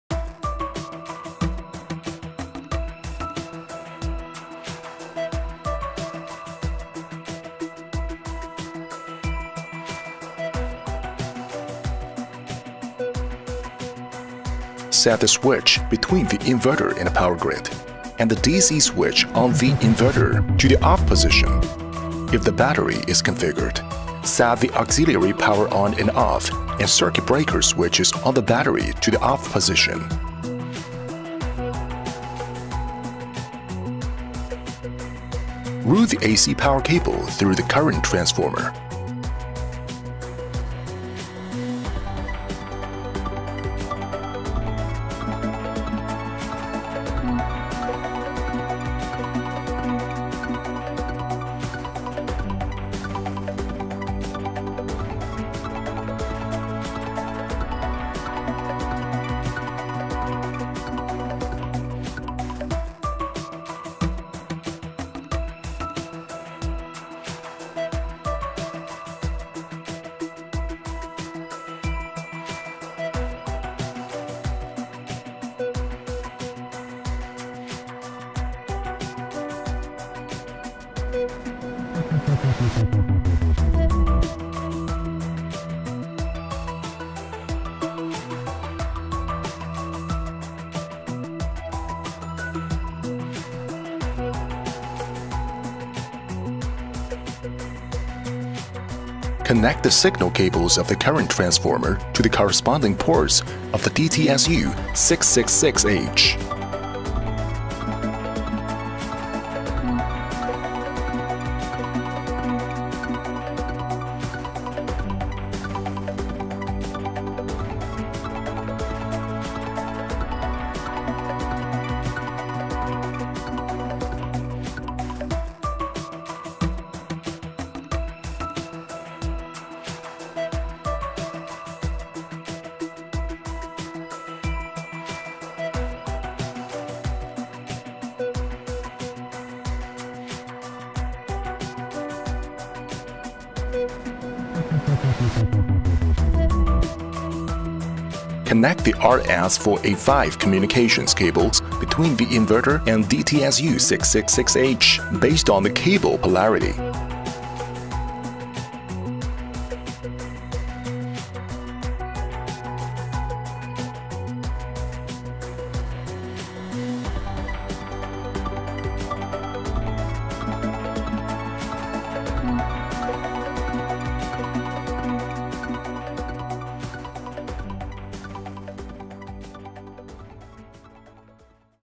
Huawei Smart Power Sensor DTSU666-H Installation Video | Mini Power
Ein Video, um zu lernen, wie man die Huawei Smart Power Sensor DTSU666-H (von Huawei) installiert.

Wenn Sie ein verwandtes Produkt bei Mini Power benötigen, können Sie auf den folgenden Link klicken: